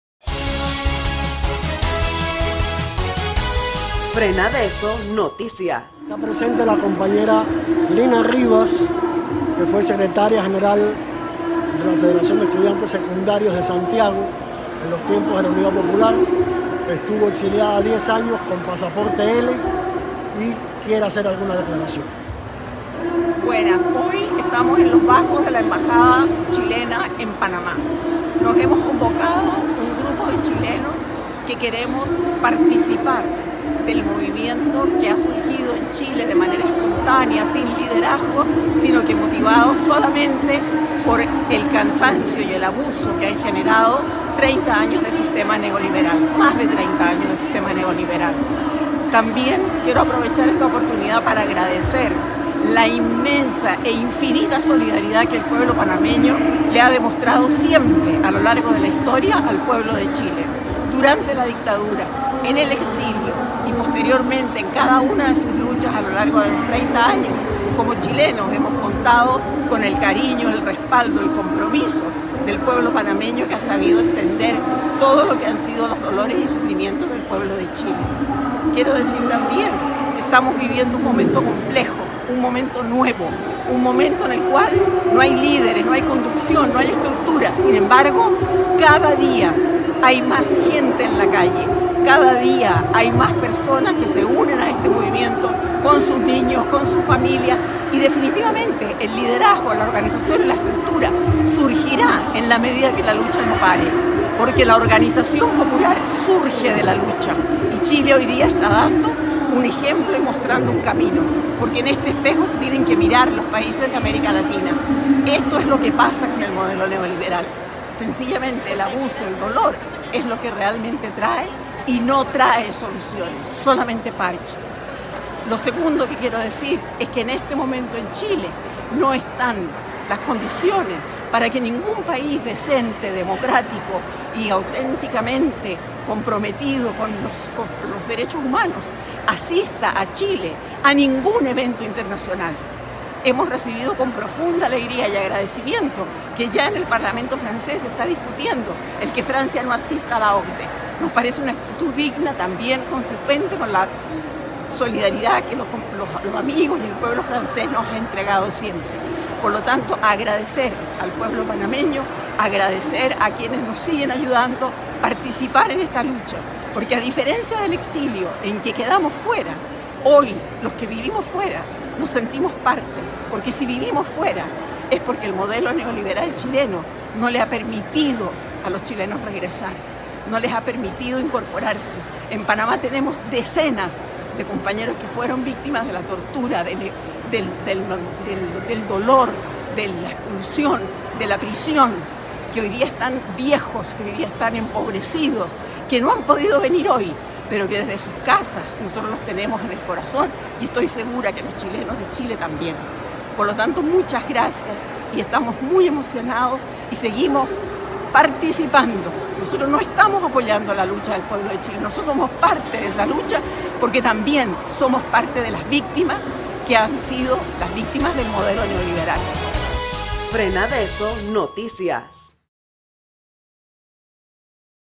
Declaraciones de Lina Rivas ex dirigente estudiantil chilena Víctima de la represión y el exilio
Lina Rivas ex dirigente estudiantil chilena.
Víctima de la represión y el exilio del régimen de Pinochet. Declaraciones desde Panamá.